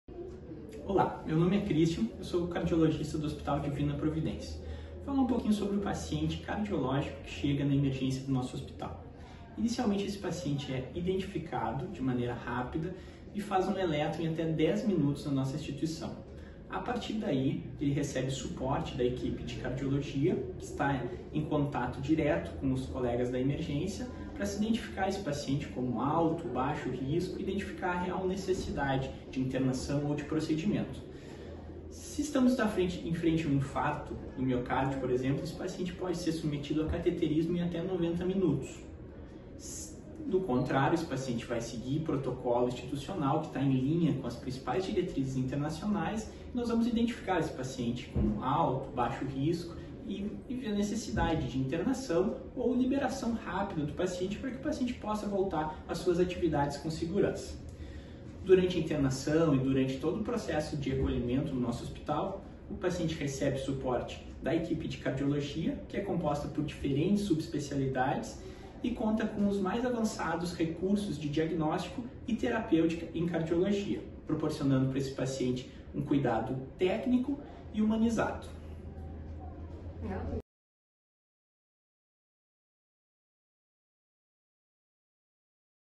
Como recebemos os pacientes com problemas cardiológicos?
O Hospital Divina Providência orgulha-se de sua capacitada equipe de cardiologistas. Eles estão sempre prontos para atender os pacientes, desde a emergência até os tratamentos mais complexos, incluindo exames e cirurgias

Assista ao vídeo para saber mais!